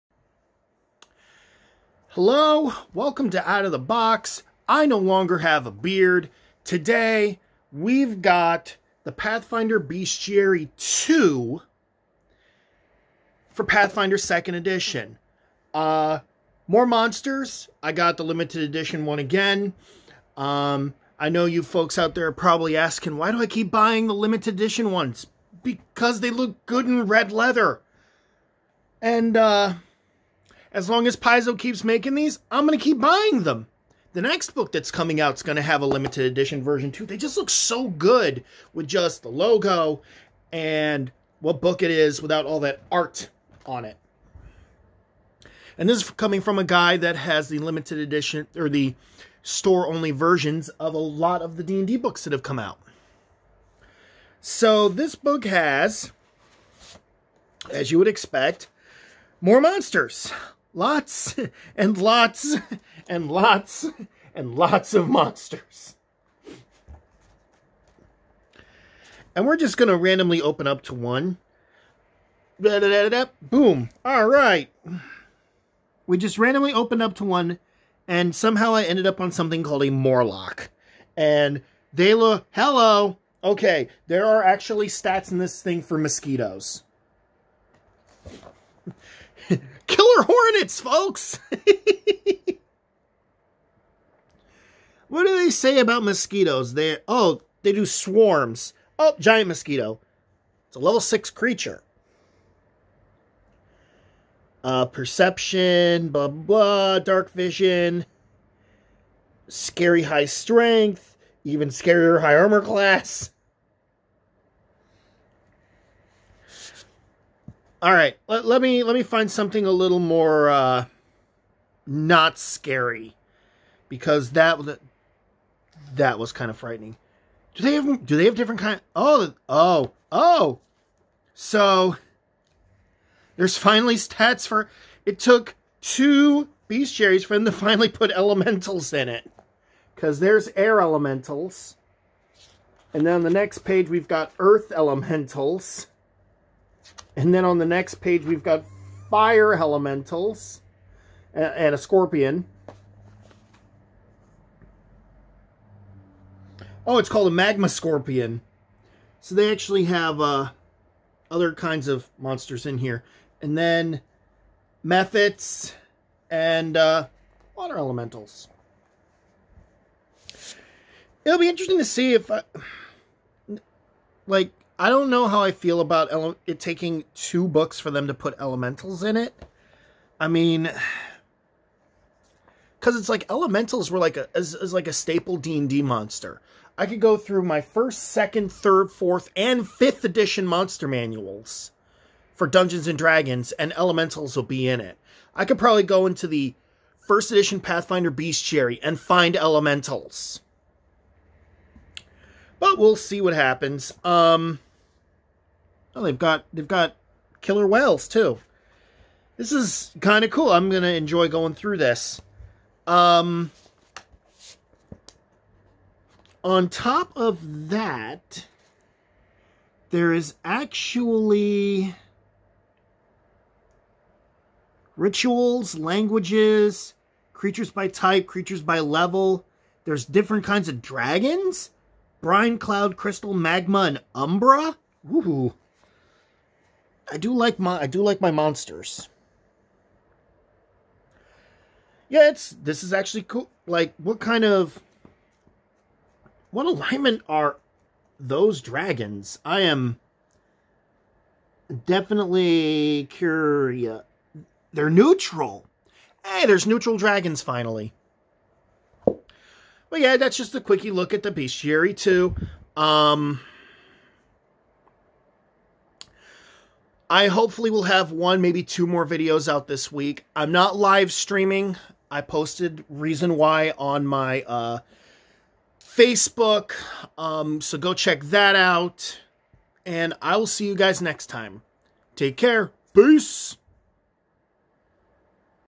Out of the Box: Pathfinder Second Edition Bestiary 2
More monsters is always a good thing, but why NOW put in iconic fantasy monsters that should have been in the first book?